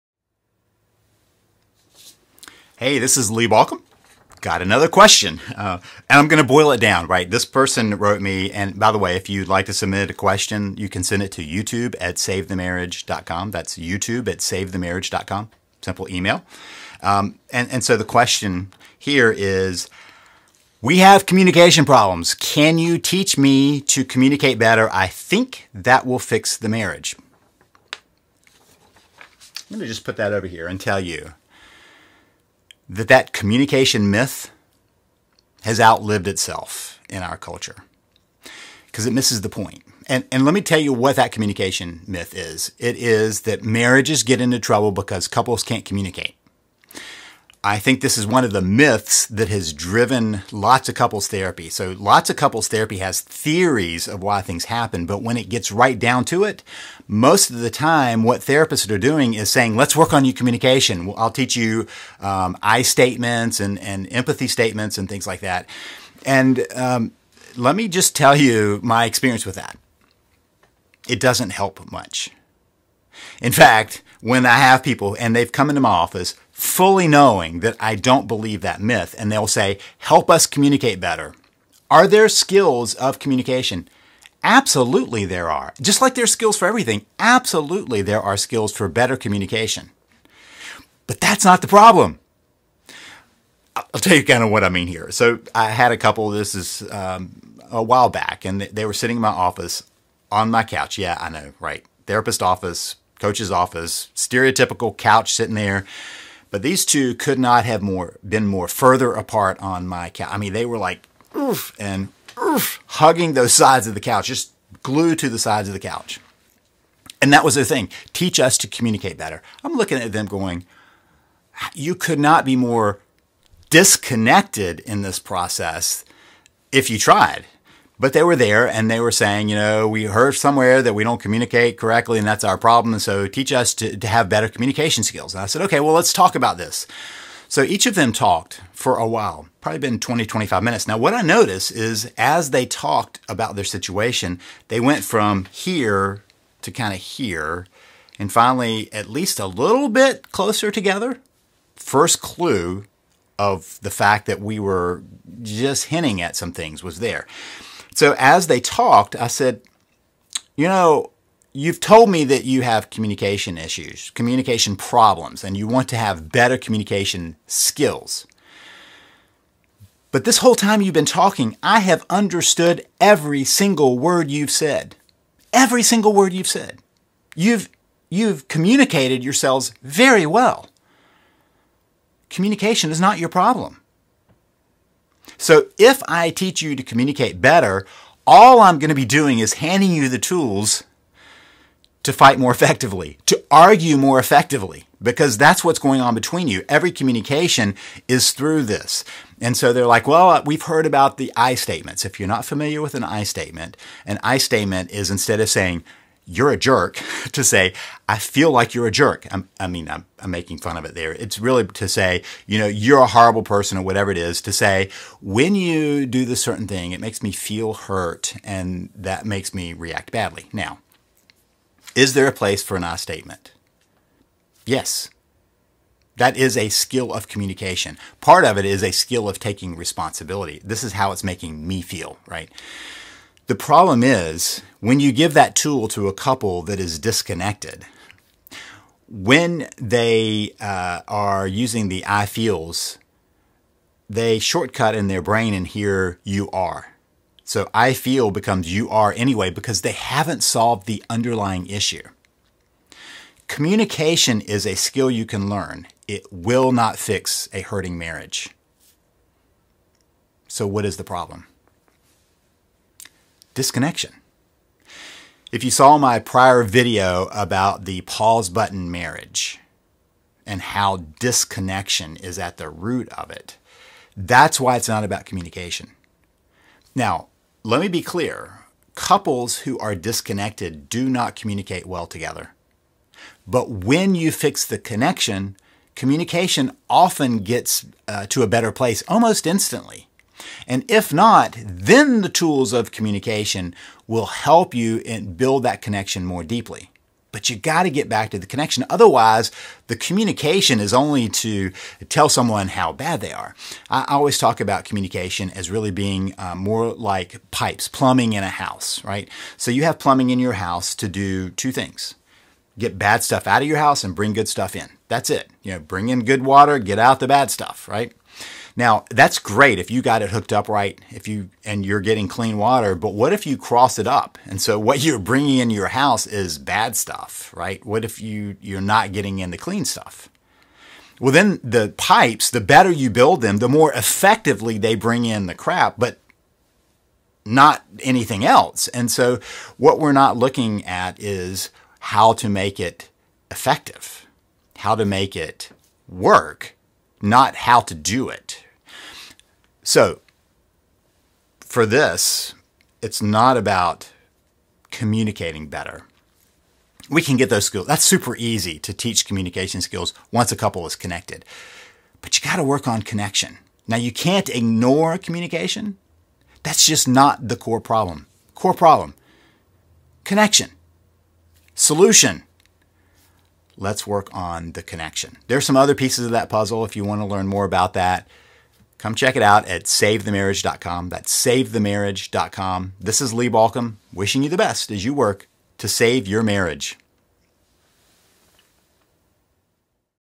Marriage Crisis? Communication is NOT the Issue!
It’s the “communication problem myth.”  I hear it over and over:  "your marriage is in trouble because you don’t have communication skills.”  Many therapists base their whole approach on helping a couple communicate better.
Which makes them much better at fighting!
Because the real issue is NOT communication.
Can you be better at communication?  Sure.  So can I… and everyone else.  But that is not why your marriage is in trouble.  Fix that, and you only get better at the delivering the poison to the relationship.  The underlying issue isn’t addressed.
And it sure isn’t fixed!
In this video, I tell you WHY communication isn’t the issue, and WHERE you should start, IF you want to rebuild your marriage and get it moving forward.